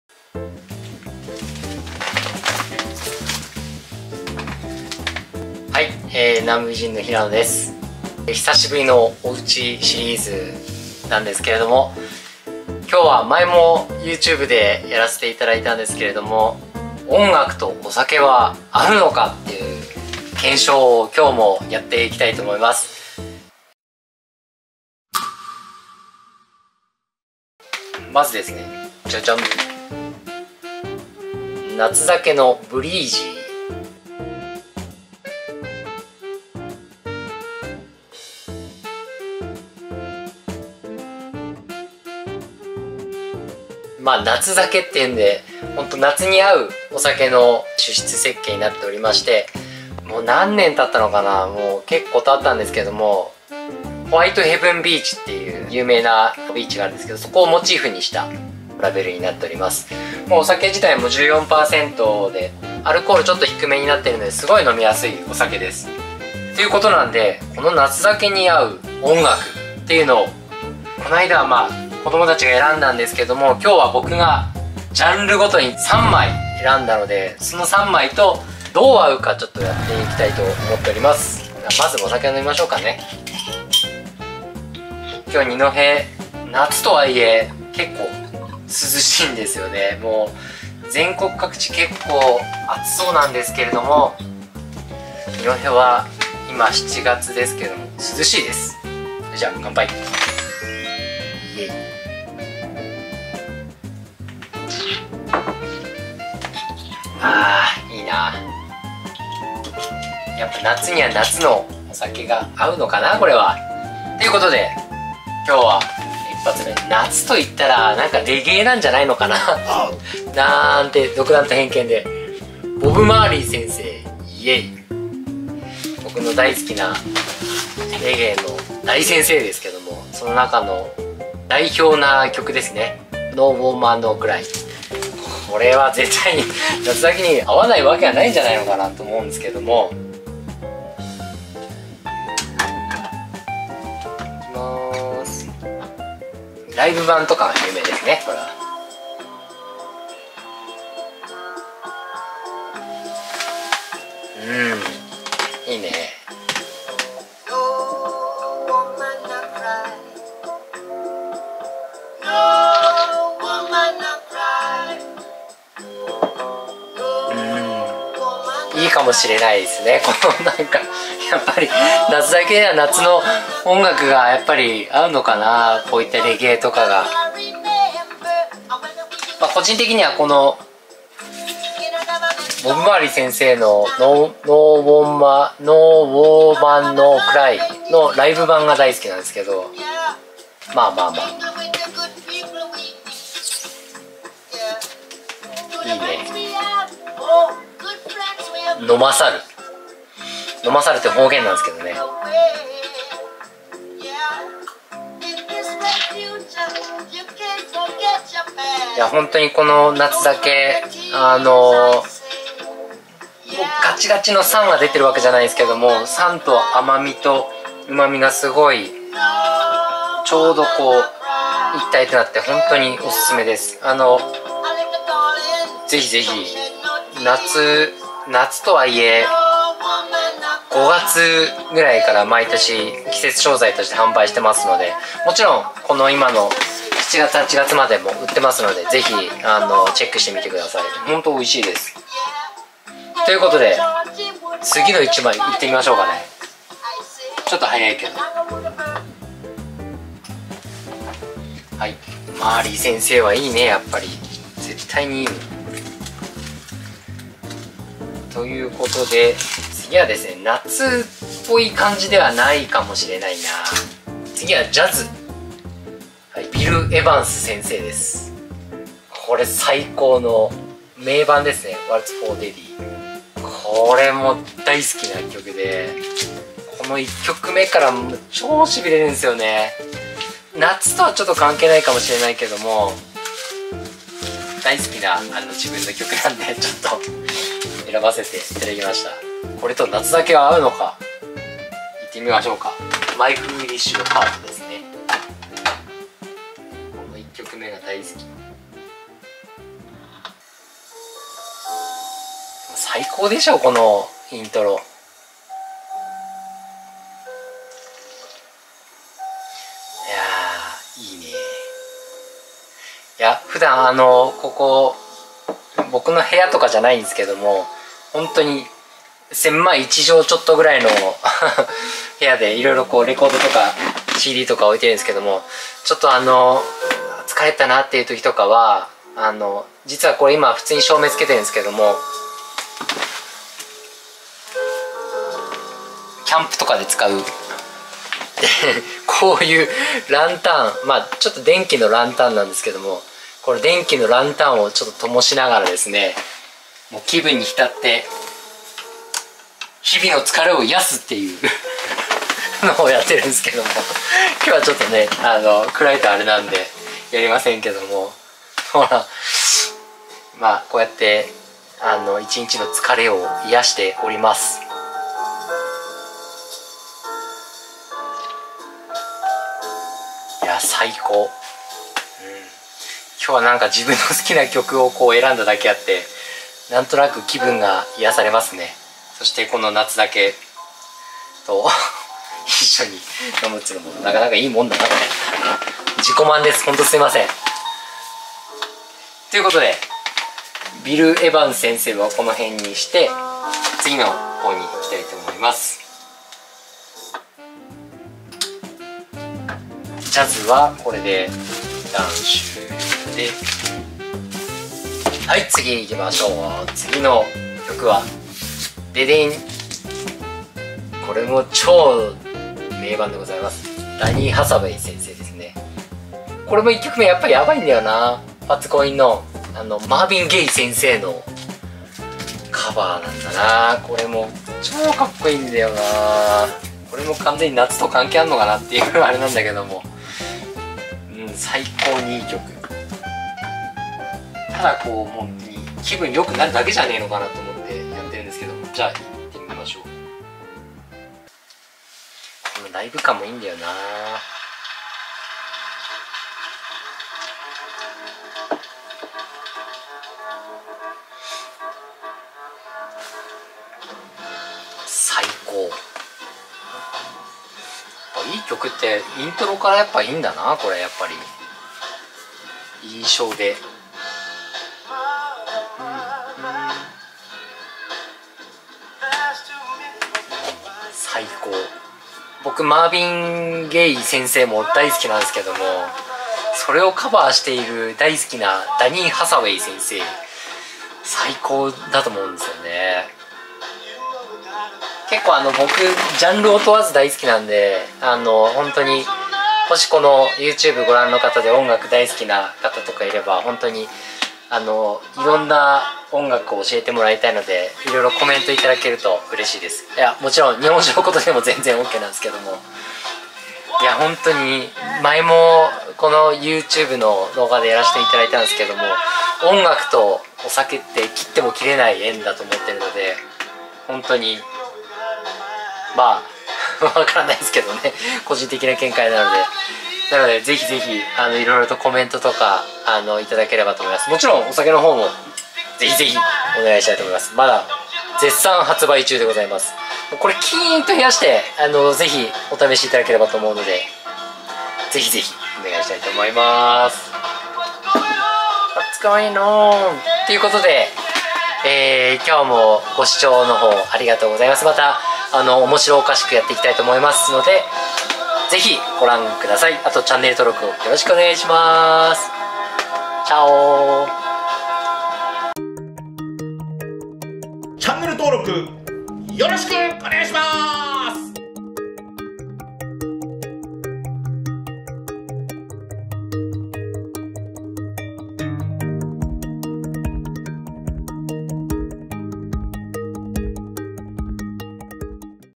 音楽とお酒を合わせてみた【おうちで南部美人】
「音楽とお酒は合うのか」第２弾ということで、前回は子供たちチョイスでしたが、今回は大好きな曲と「夏酒 Breezy」を合わせていきたいと思います。好きな曲と好きなお酒なので合わないわけがないと思うんですけどね。検証の方是非ごらんください！

毎週土曜日の18：00くらいに配信しています。

▽自社サイト▽
南部美人 コーポレートサイト

▼制作▼
株式会社カルティブ